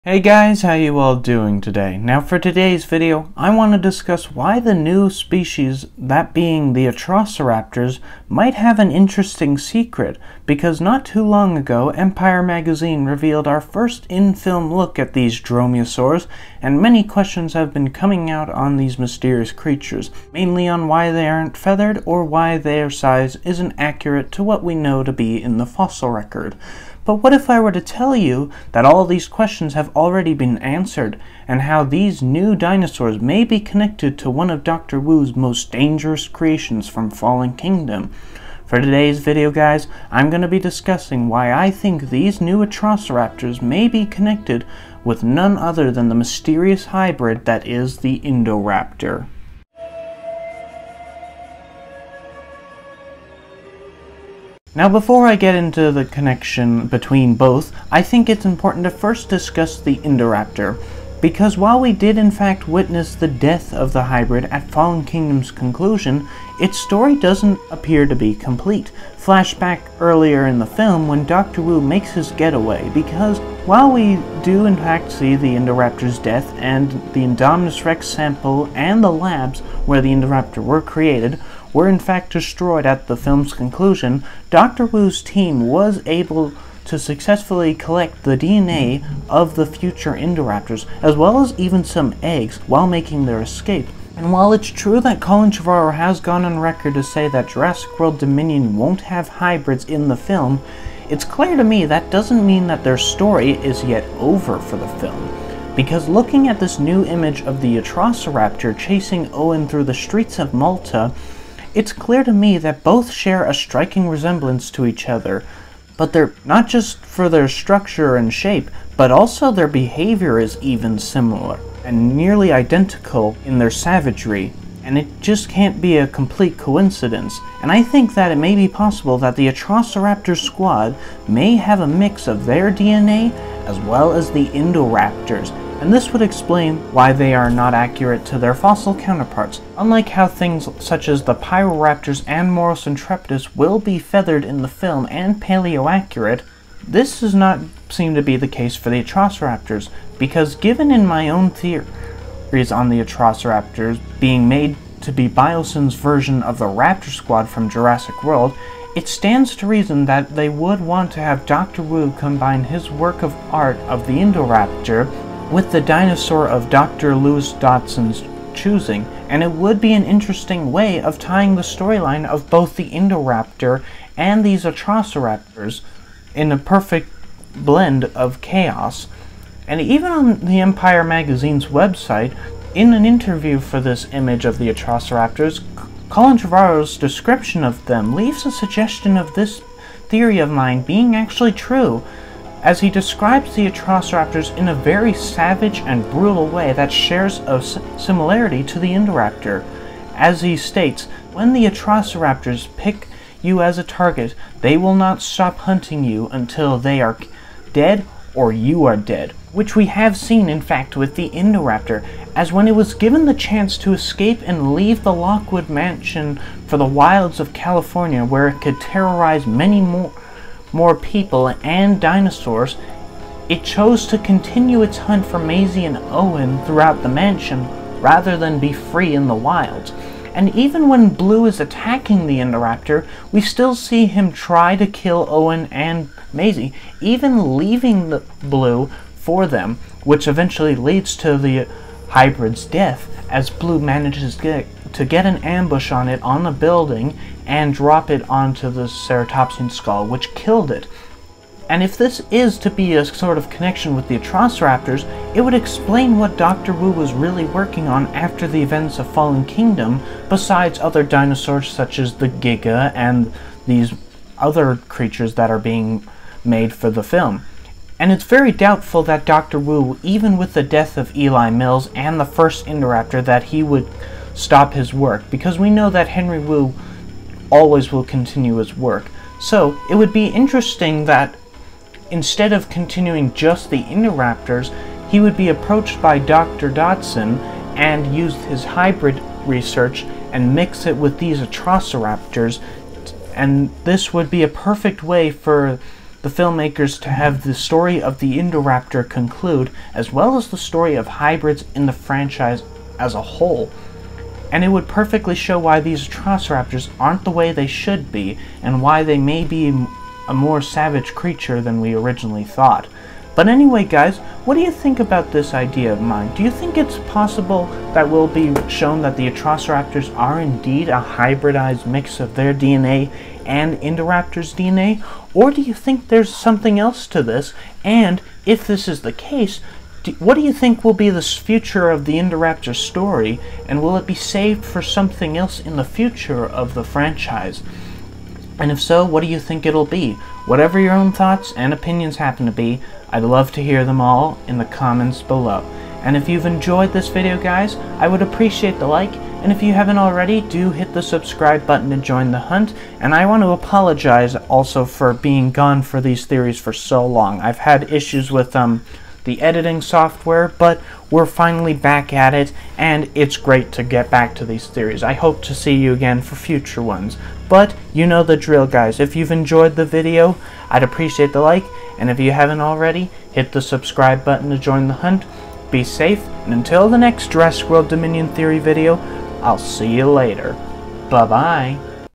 Why The Atrociraptors May Be Connected To The Indoraptor | Jurassic World: Dominion
With many questions about the appearance of the new Atrociraptor being in the air lately I discuss how we may have always had the answers leading to Dr. Wu’s Indoraptor